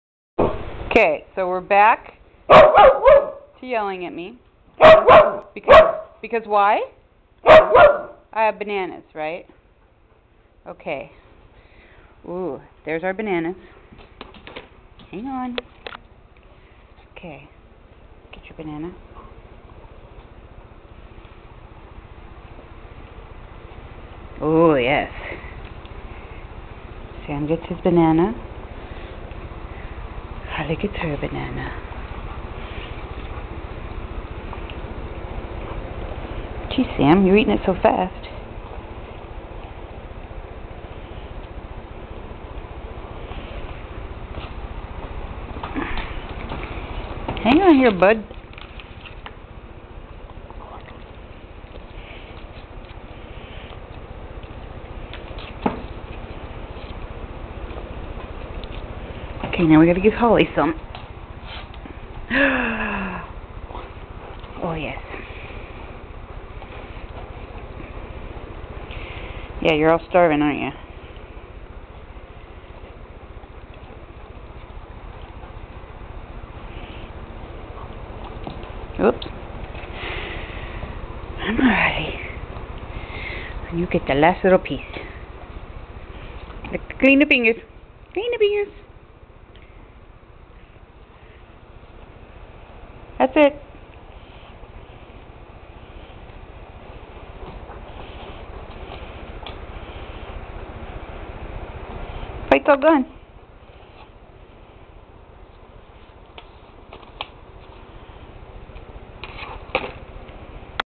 Sam and Holly Banana
Sam and Holly eat a banana